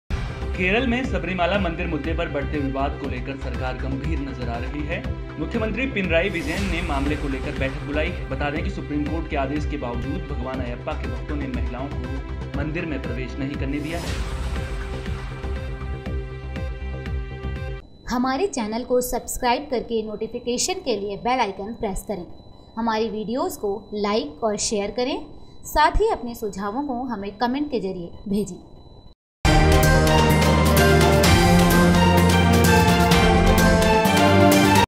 GSN NEWS: सबरीमाला विवाद को लेकर सीएम पिनराई विजयन ने बुलाई बैठक
केरल में सबरीमाला मंदिर मुद्दे पर बढ़ते विवाद को लेकर सरकार गंभीर नजर आ रही है...मुख्यमंत्री पिनराई विजयन ने मामले को लेकर बैठक बुलाई है..बता दें कि सुप्रीमकोर्ट के आदेश के बावजूद भगवान अयप्पा के भक्तों ने महिलाओं को मंदिर में प्रवेश नहीं करने दिया है।